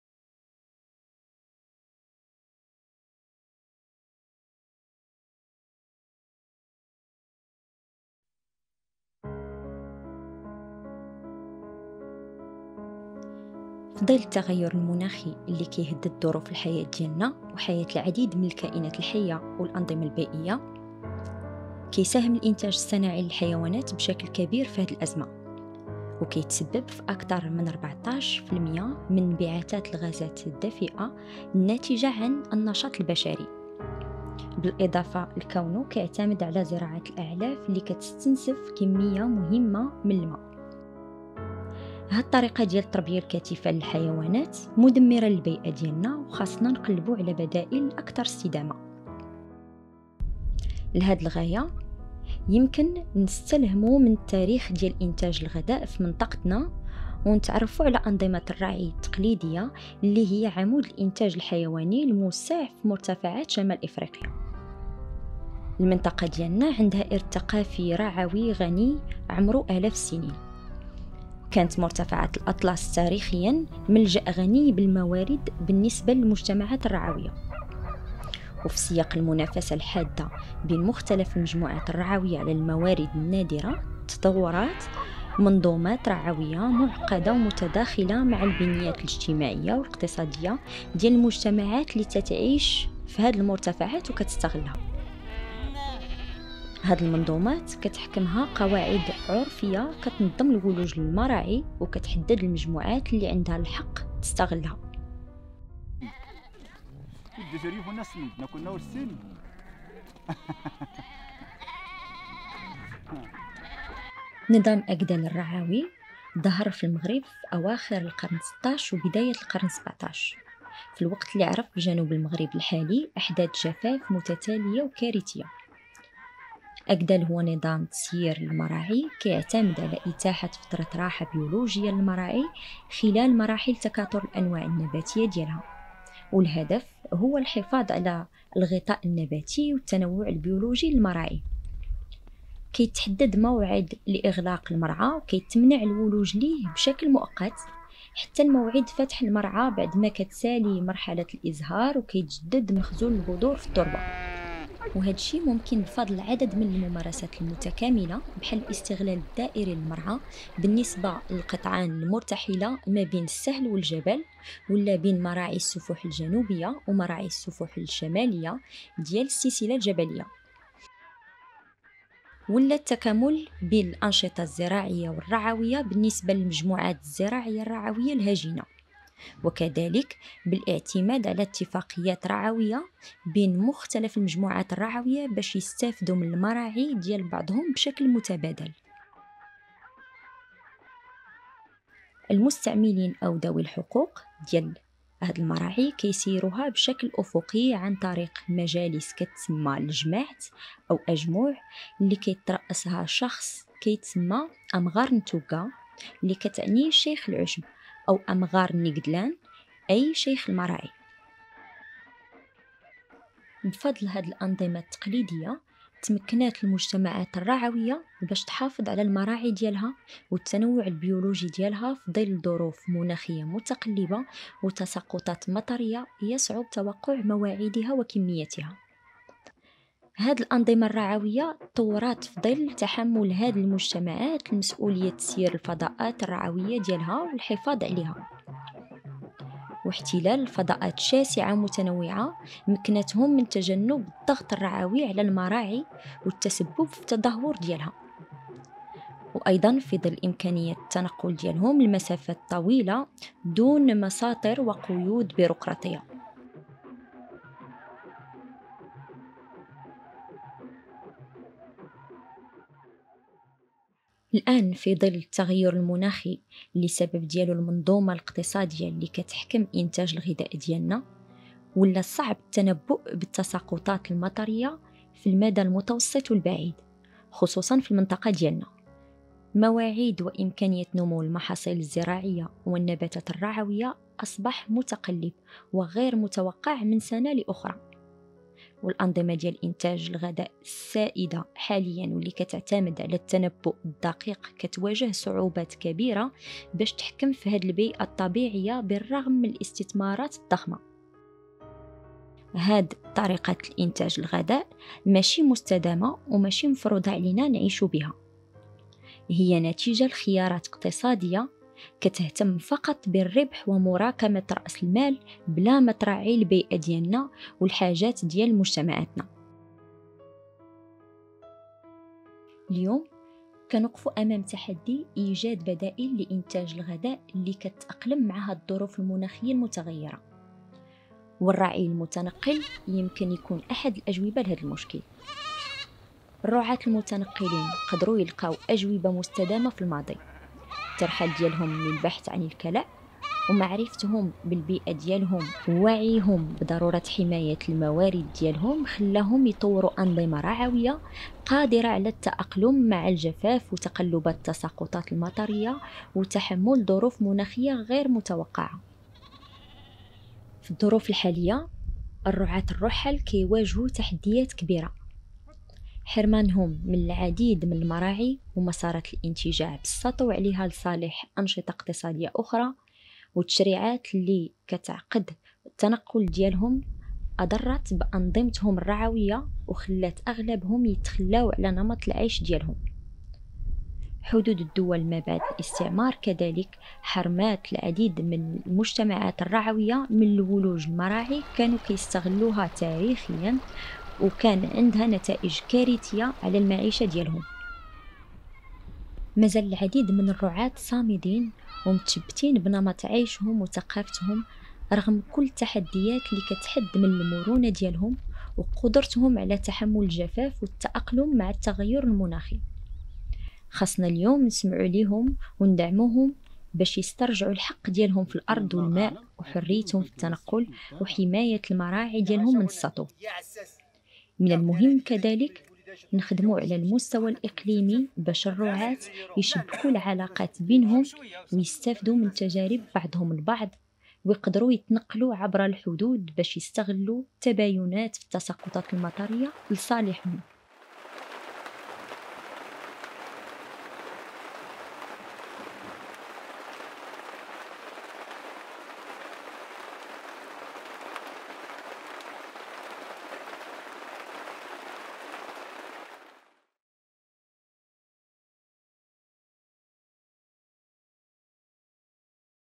الشريط الوثائقي: الرعي المٌتنقل ومستقبل السيادة الغذائية
في ظل التغير المناخي الذي سببته المنظومة الاقتصادية التي تحكم إنتاج غذائنا، أصبح من الصعب التنبؤ بالتساقطات المطرية في المدى المتوسط والبعيد، خصوصا في منطقتنا. مواعيد وإمكانيات نمو النباتات الرعوية والمحاصيل الزراعية أصبح متقلبا وغير متوقع من سنة لأخرى. نقف اليوم أمام إيجاد بدائل لإنتاج الغذاء تتأقلم مع هذه الظروف المناخية المتغيرة، والرعي المتنقل يمكن أن يقدم بعض الإجابات. يكتشف هذا الشريط الأنظمة الرعوية التقليدية التي تطورت في مرتفعات شمال إفريقيا خلال القرون الماضية، والتي استطاعت إيجاد أجوبة مستدامة لإنتاج الغذاء تراعي بيئتنا وتحفظ سيادتنا الغذائية.